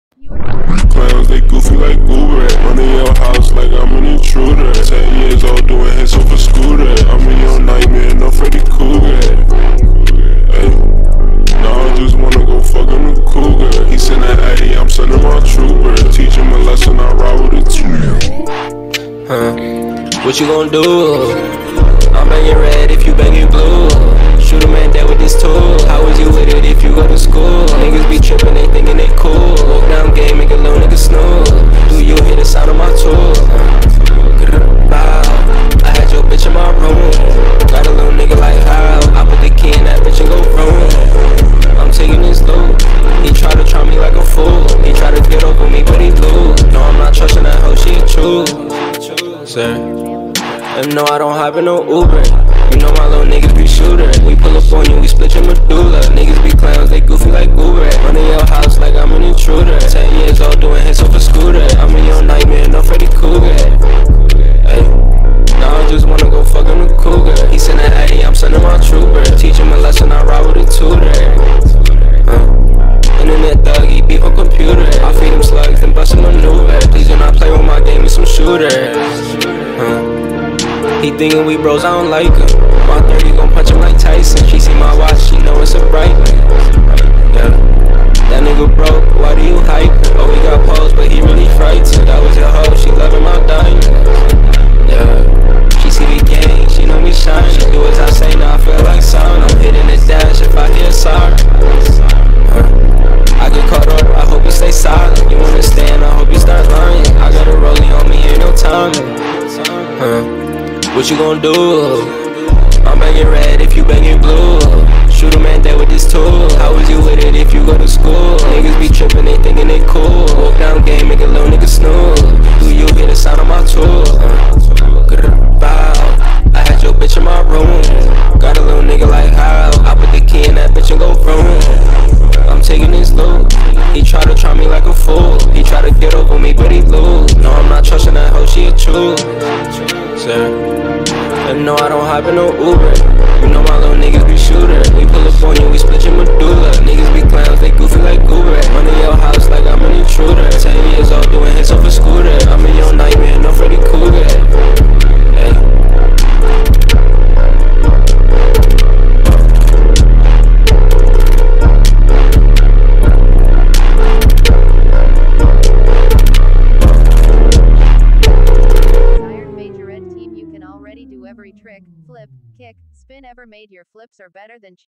majorette subliminal 🤍
you are the best majorette dancer to ever live
you dance so good you put the best dancer in the world to shame
omg your a better dancer that the dancing dolls
your already on your desired majorette team
you can already do every trick,flip,kick,spin ever made
your flips are better than cheerleaders
your so flexible you could fold into a pretzel
your immune to getting injured in any type of way
your stamina is so high you can dance for years
your moves are so sharp and on point
your dance team is full of amazing dancers
everyone on your dance team gets along with you
your coach is so nice to everyone
everyone on your team is always in sync
manifest team jackets,shirts,pants,bows,bookbags,duffelbags,
manifest your own infinity bookbag with your name on it
your infinity bookbag is immune to getting damaged or stolen or lost
manifest a team jacket with your name on ityour family is rich
your family can pay your majorette fees
manifest makeup,wigs